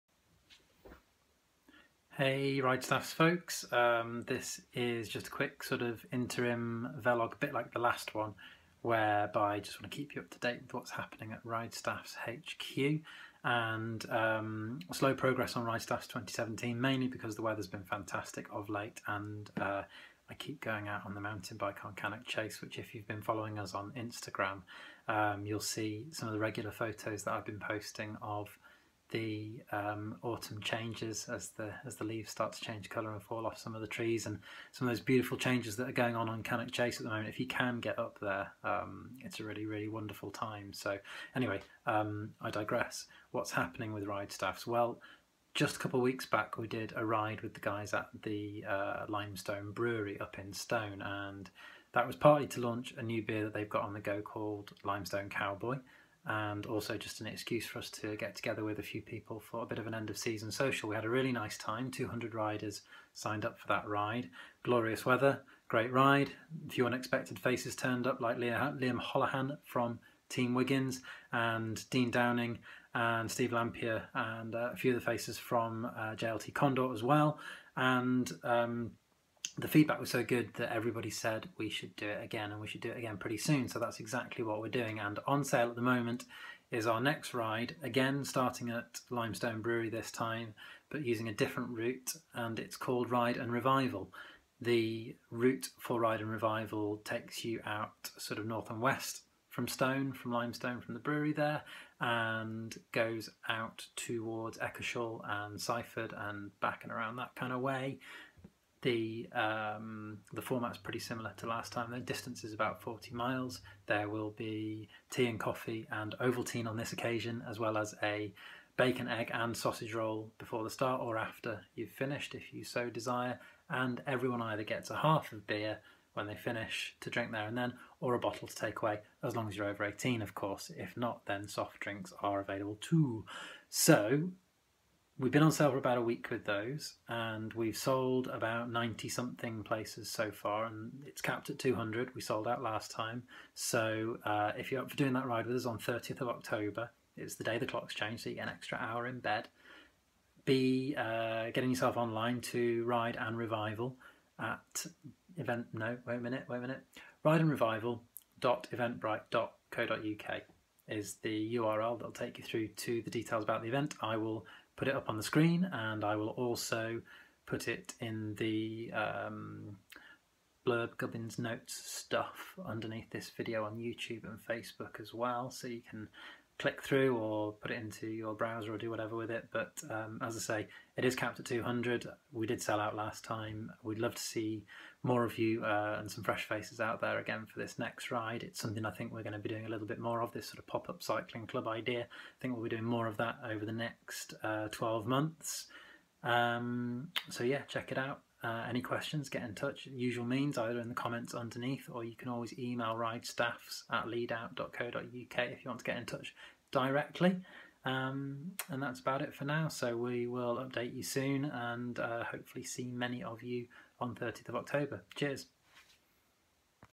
velog #20 - latest from RideStaffs on 11.10.16
More from ridestaffs including details of our latest event with Lymestone Brewery, the Ride & Revival ride.